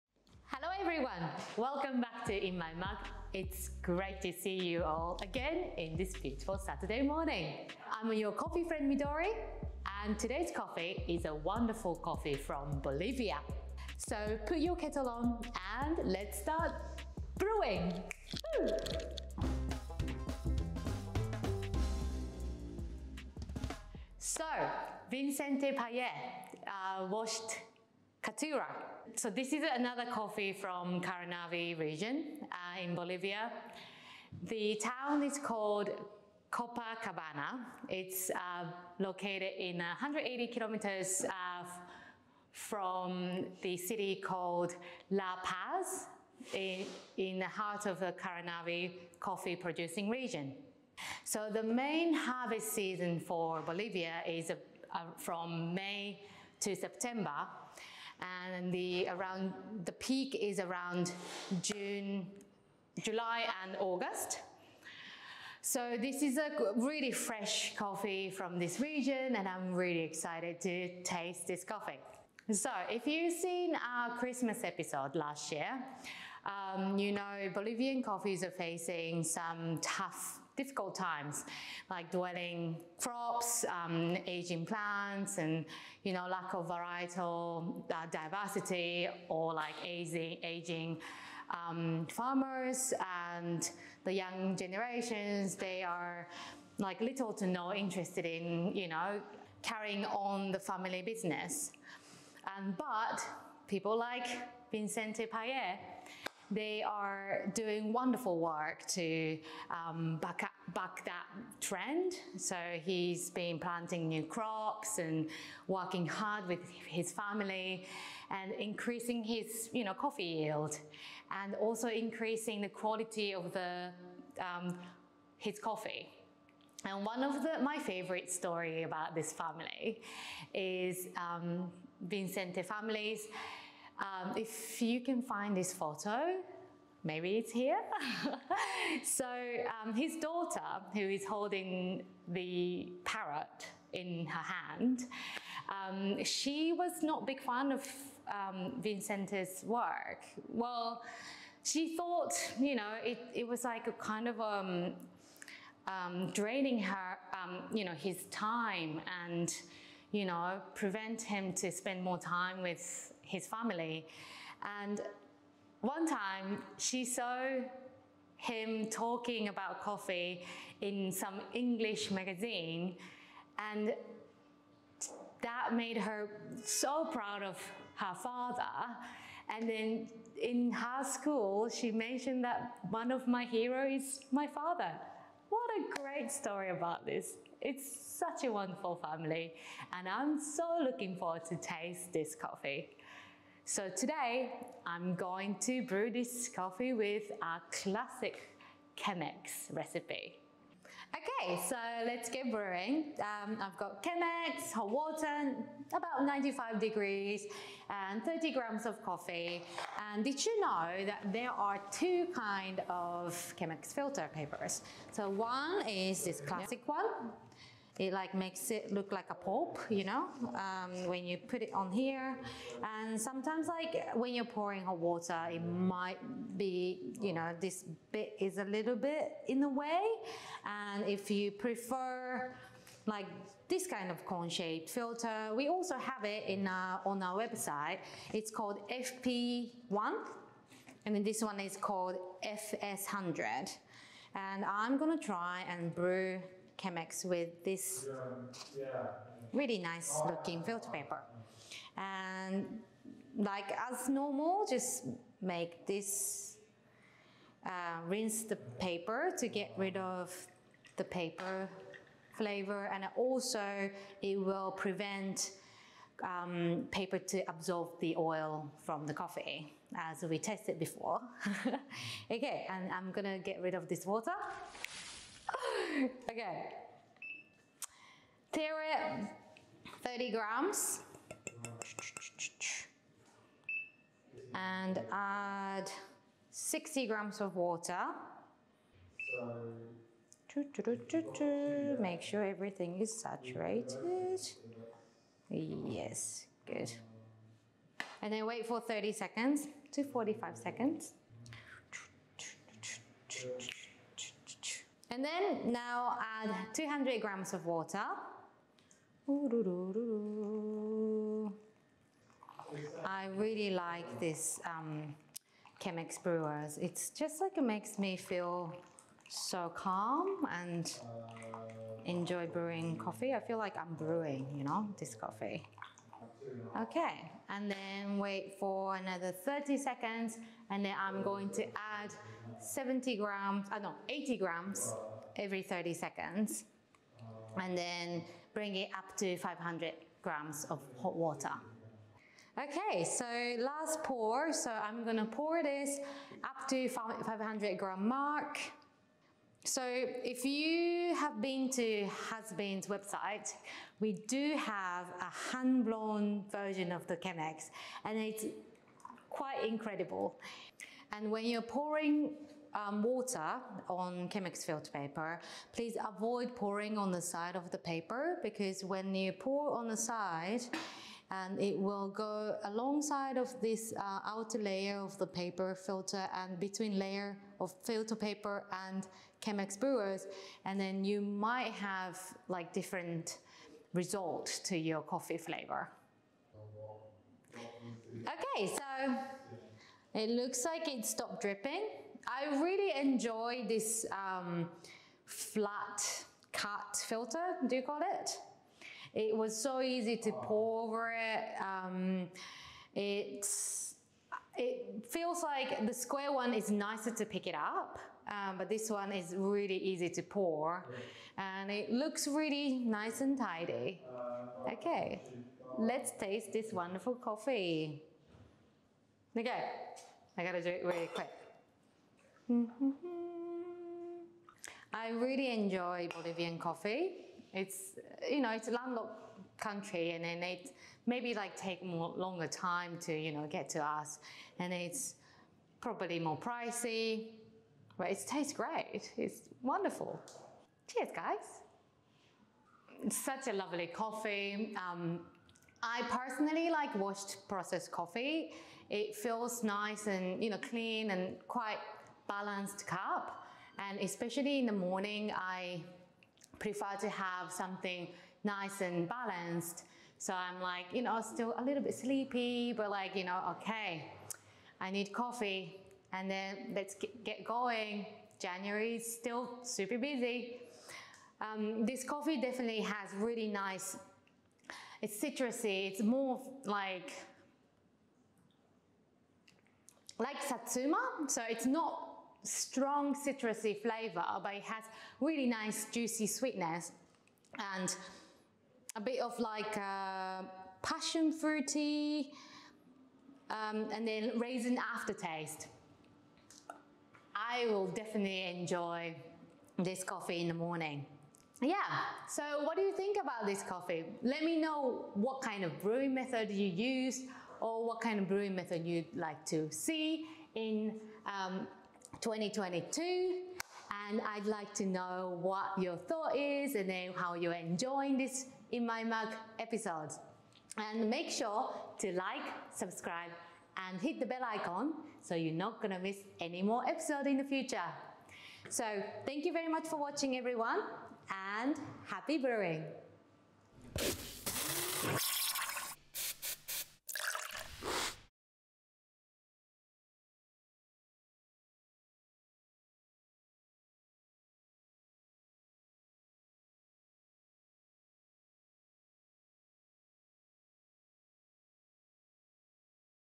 In My Mug 688: Bolivian coffee brewed by Chemex 🇧🇴 | HASBEAN
We're once again in Caranavi this week on InMyMug with another wonderful coffee from Bolivia!

Your subscription coffee for this week is a washed processed mix of red and yellow Caturra from Vincent Paye, grown in the Yungas region of Caranavi, Bolivia.

Also make sure to check out our Hasbean Chemex brew guide

🤤 Tasting notes: Satsuma, passionfruit, toffee, raisin